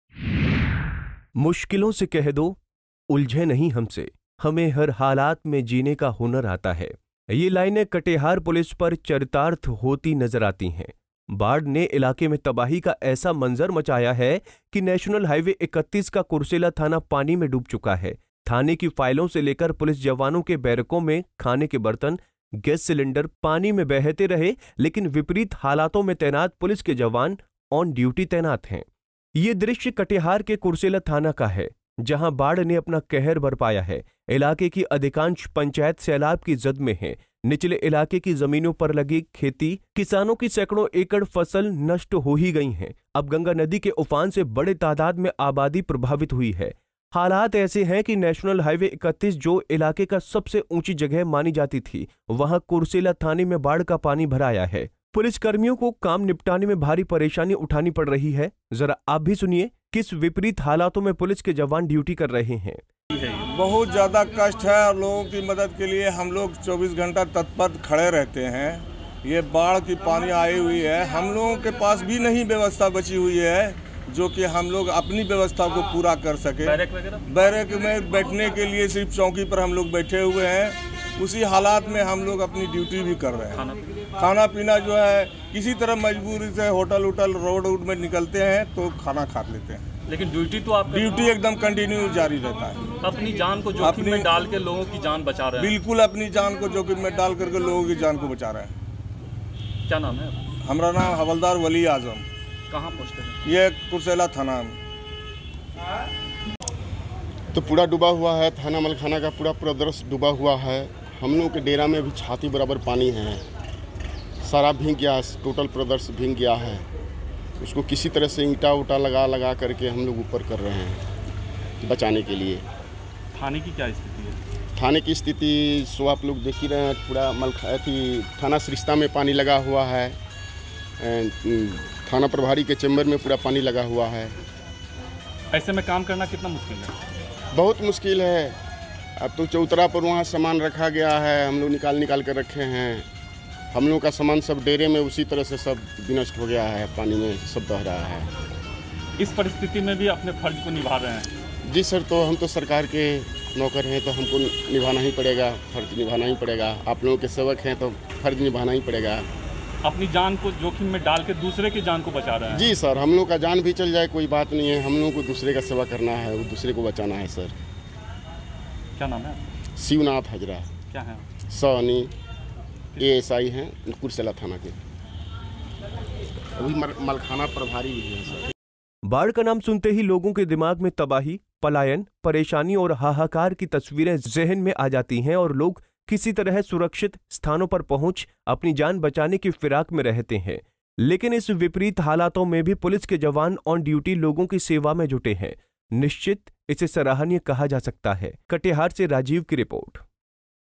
Katihar: बाढ़ की तबाही में भी विपरीत हालातों में पुलिस के जवान "ऑन ड्यूटी" तैनात
KatiharPolice # Flood

"मुश्किलों से कह दो उलझे नहीं हमसे , हमें हर हालात में जीने का हुनर आता हैं" ये लाइनें कटिहार पुलिस पर चरितार्थ होता नजर आती हैं ! बाढ़ ने इलाके में तबाही का ऐसा मंजर लाया हैं कि नेशनल हाइवे - 31 का कुर्सेला थाना पानी में डूब गया ! थाने की फाइलों से लेकर पुलिस जवानों के बैरकों में खाने के बर्तन , गैस सिलिंडर पानी में बह रहे लेकिन विपरीत हालातों में पुलिस के जवान "ऑन ड्यूटी" तैनात हैं